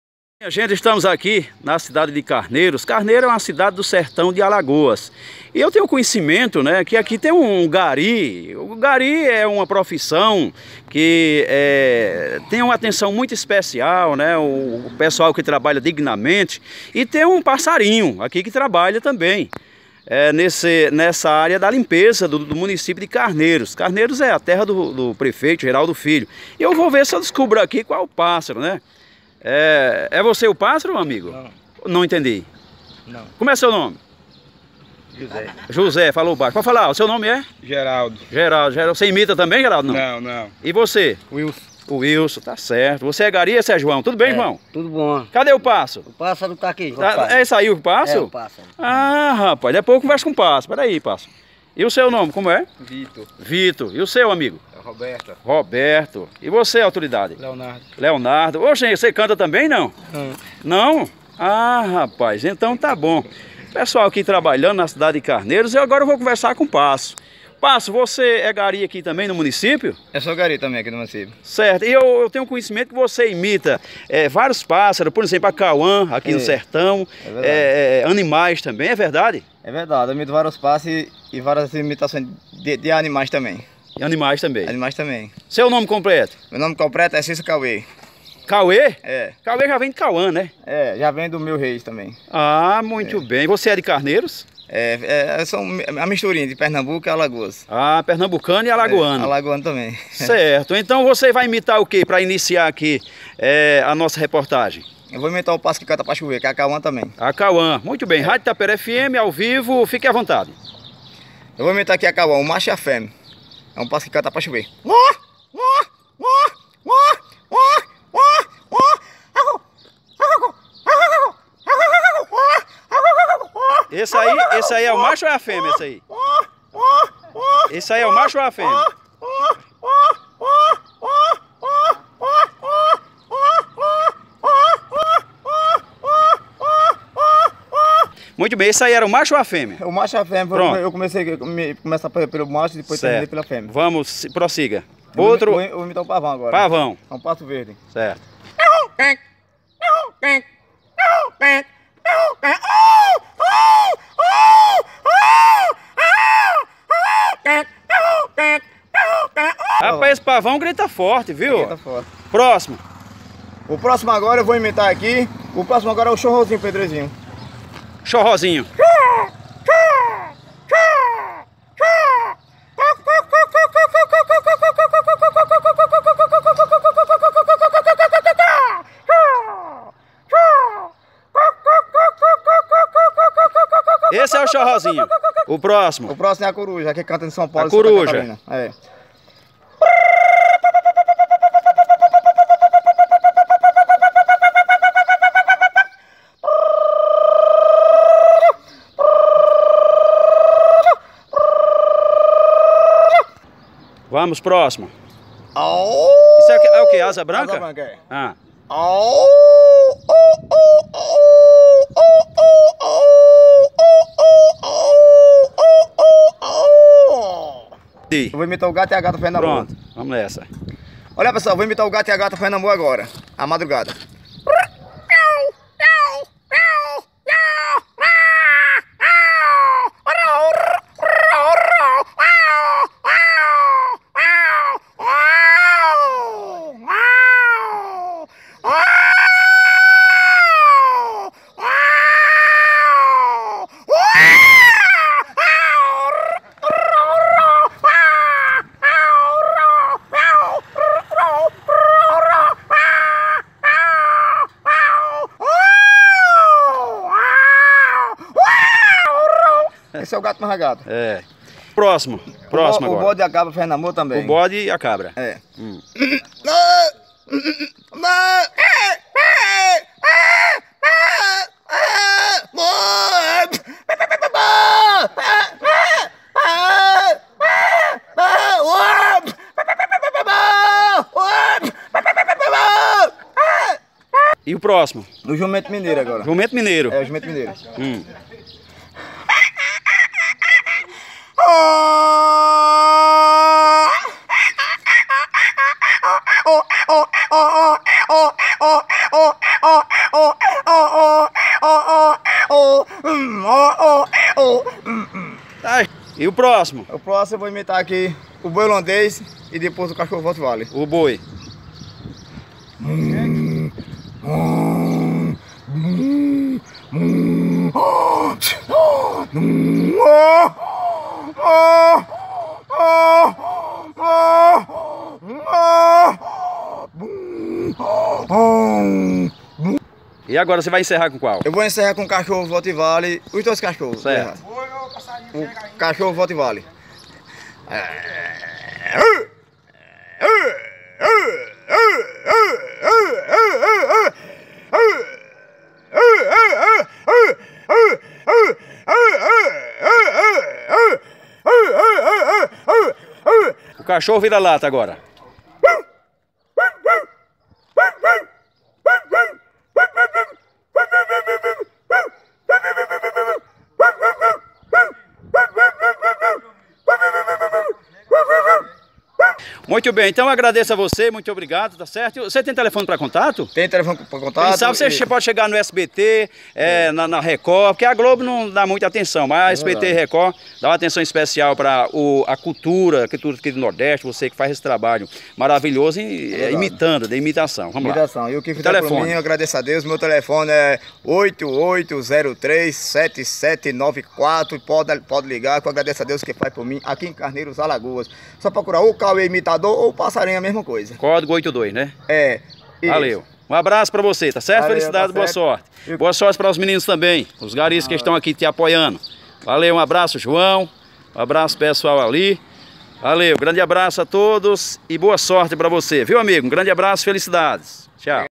Maior Imitador de pássaros e Animais, Sertão de alagoas.
Vídeos produzidos pela equipe da Tapera FM, situada em São José da Tapera, Sertão de Alagoas. Trata-se de conteúdos culturais e educativos